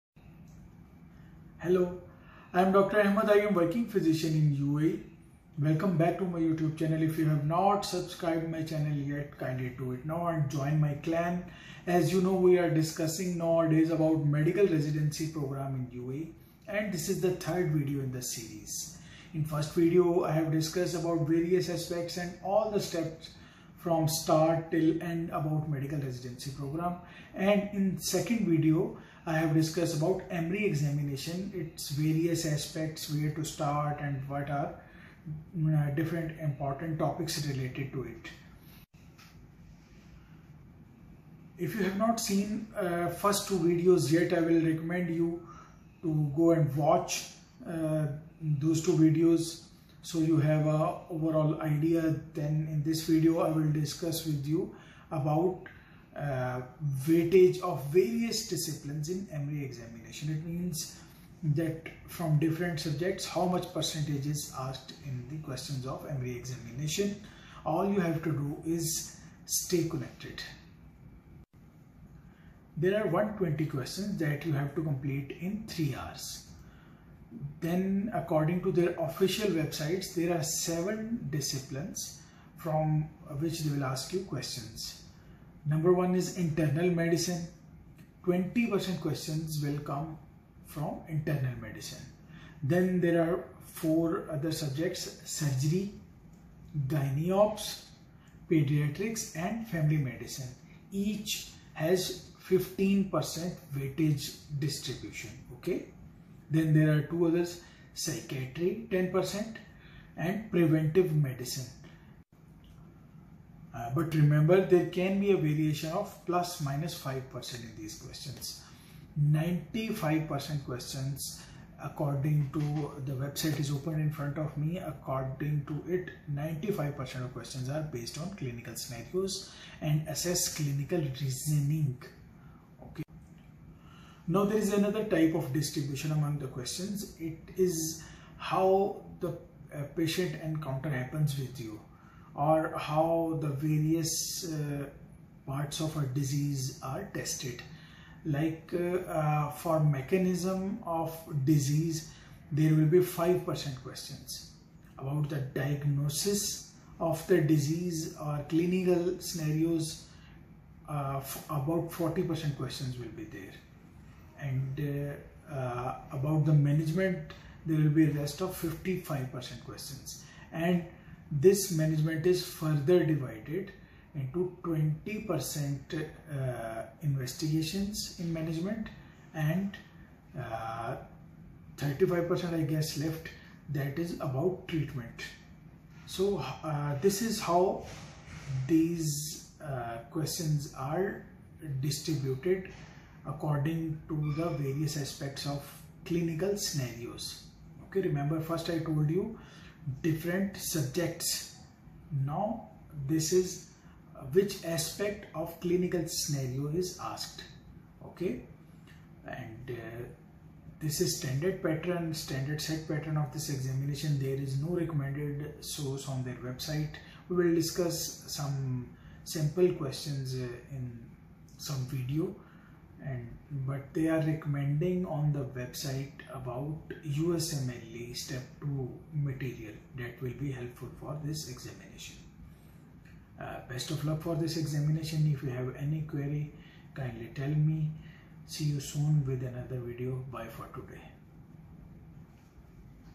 EMREE / Subjects distribution / type of Questions/ video 3 of Medical Residency Program UAE
This is Third video in my medical residency program series. this about EMREE exam again, how various subjects are distributed in exam and what type of questions are asked in exam.